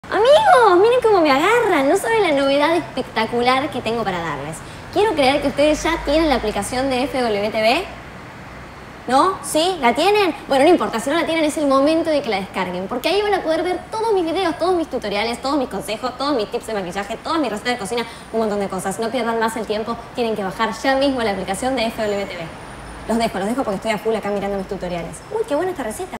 Ganá con FWTV - Aplicación Móvil
¡Escuchá bien y hacé como Cande Molfese que ya se bajó la app de FWTV! Ya podés descargarla! ¿Qué esperas?